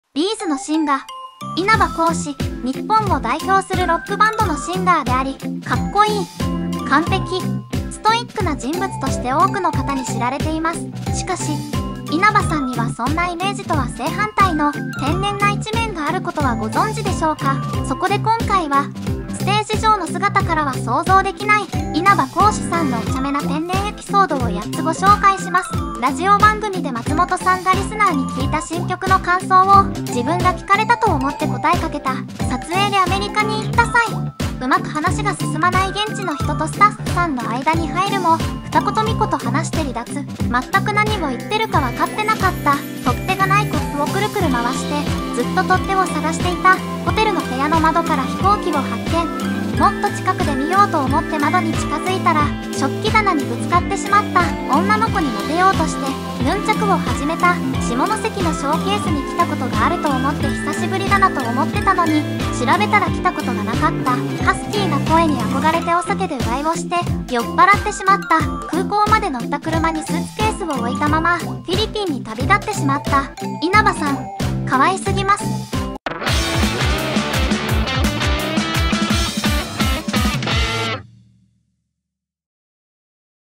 【B’z】クスッと笑える！稲葉浩志のお茶目な天然エピソード8選
VOICE VOX:春日部つむぎ